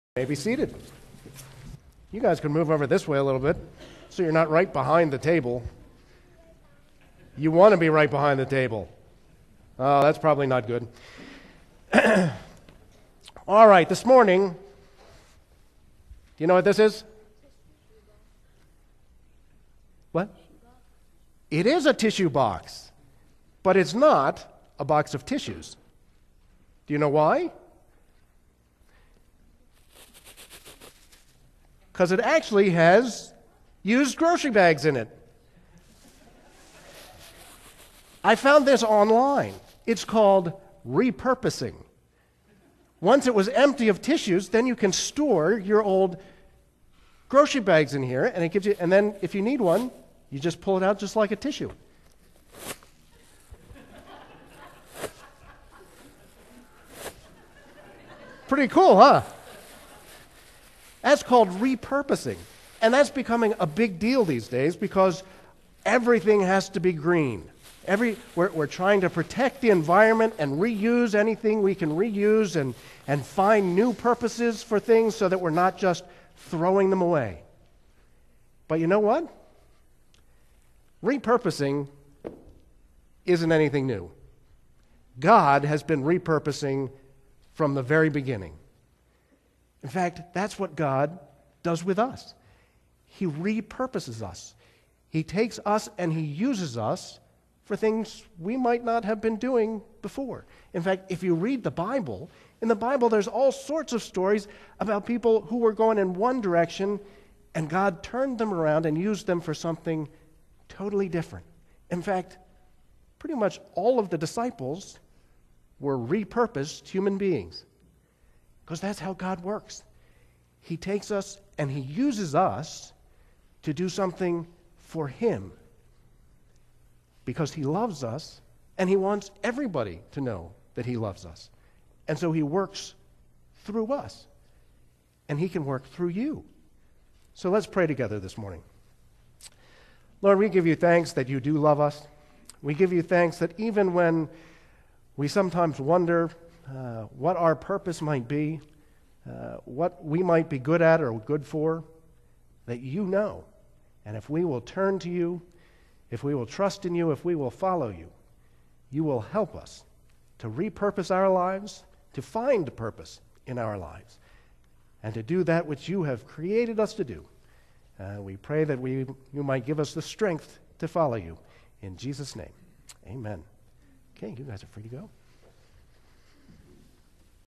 Sharing with Our Children 1/26/14
Rev. Doug McPheeters gives the children's sermon during the Sunday, January 26, 2014 Worship Service at Middletown Presbyterian Church, Media, PA.

Middletown Presbyterian Church
273 S Old Middletown Road
Media, PA  19063